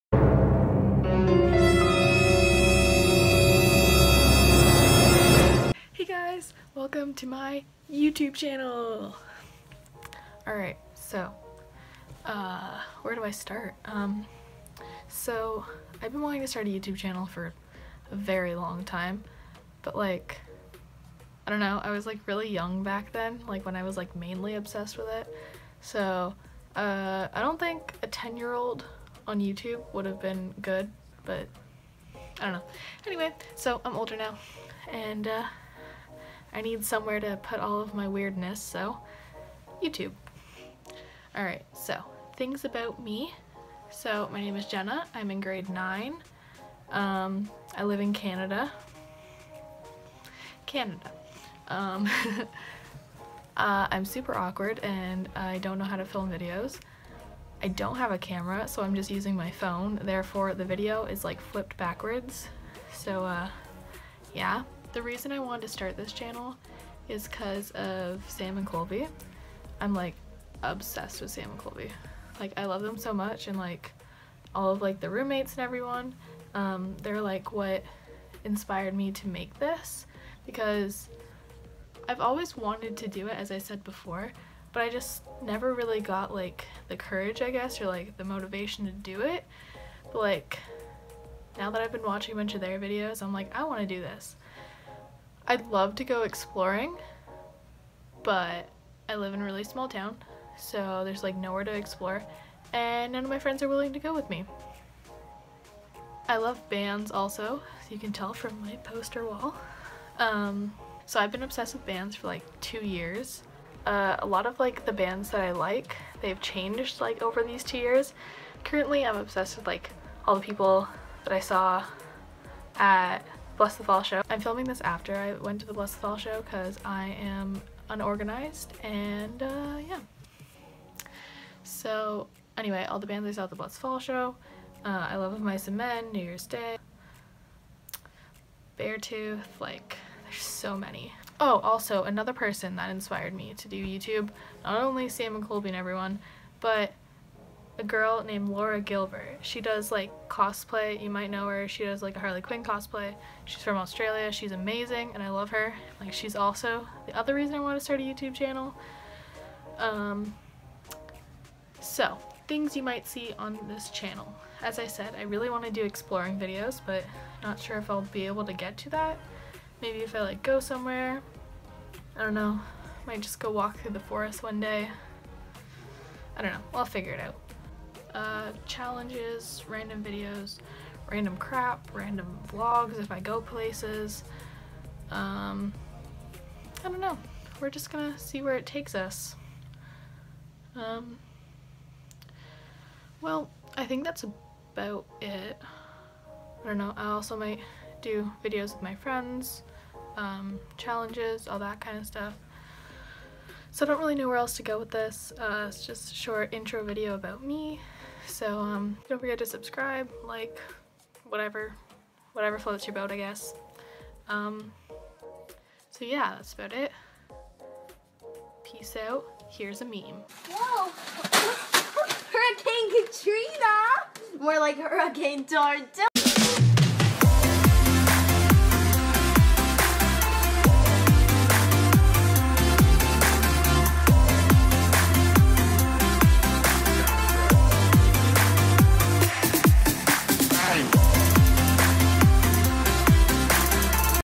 INTRO VIDEO | NEW CHANNEL
Hey guys welcome to my new channel! Sorry about the video being so quiet, but I'm super awkward. I promise the videos will be better eventually!